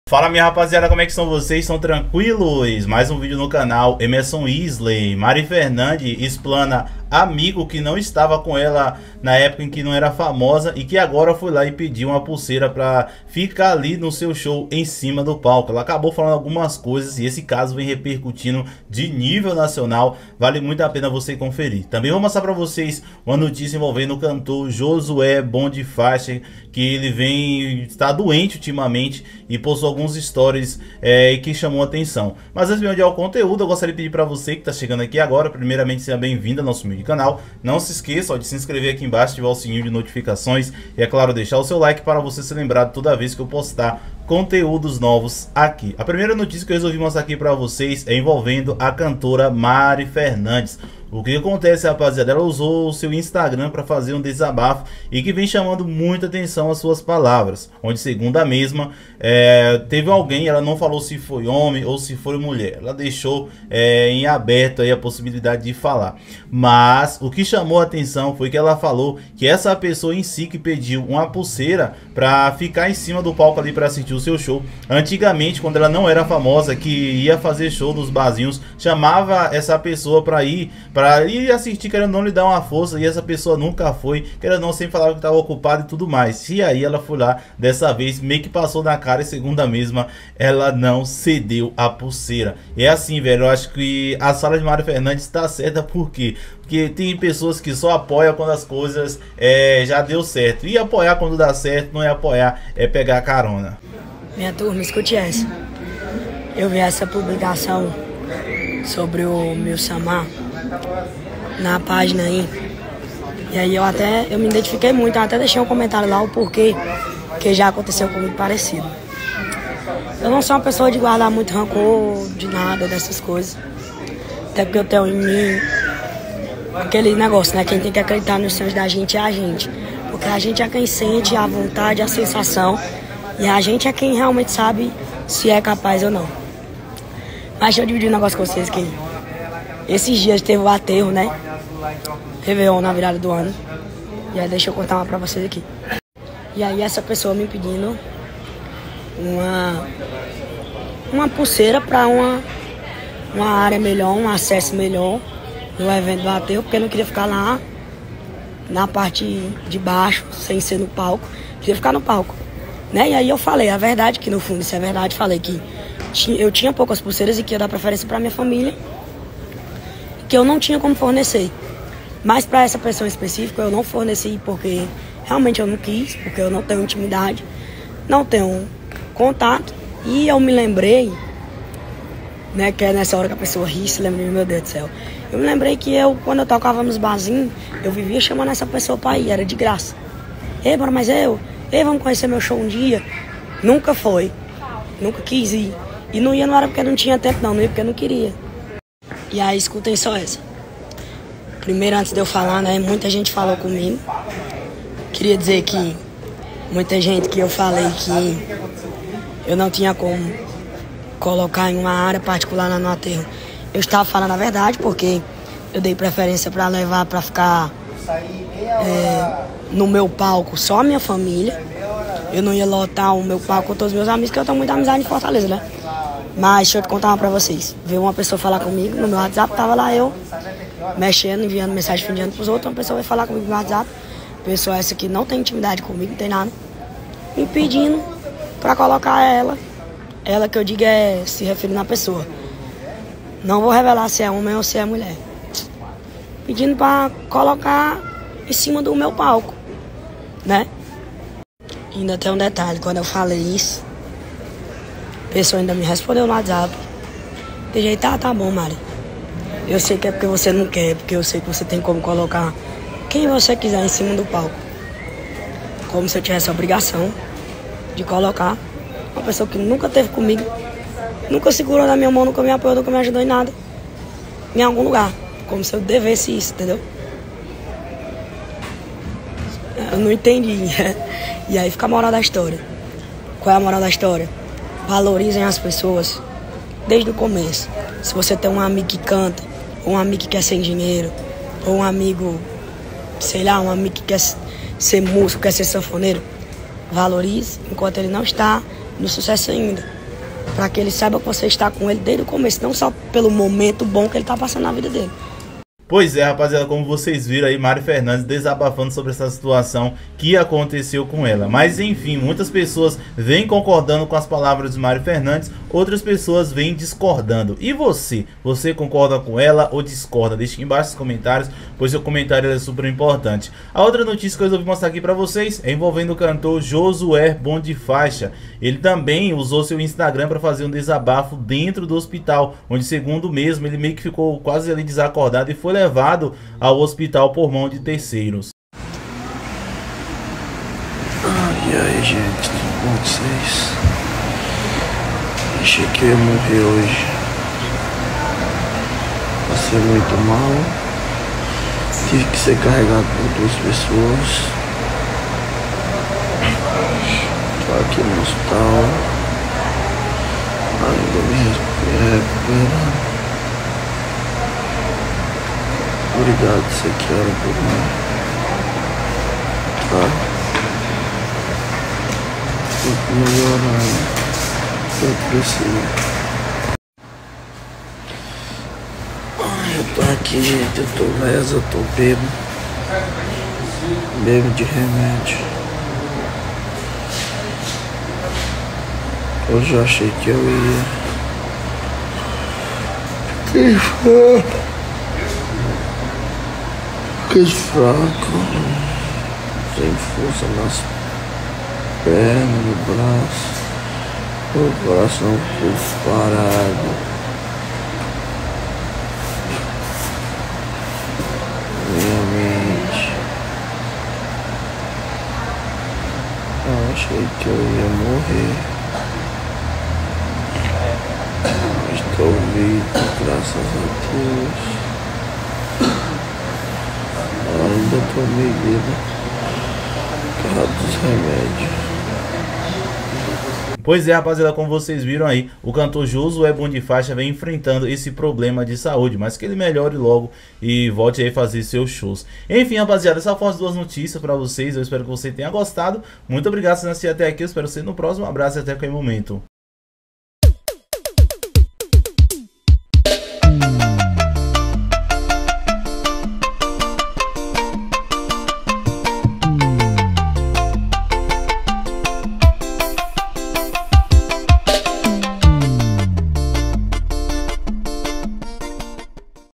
MARI FERNANDEZ EXPLANA "AMIGO" SÓ QUER COLAR NA HORA DA FAMA- JOSUÉ BOM DE FAIXA DOENTE
MARI FERNANDEZ EXPLANA AMIGO!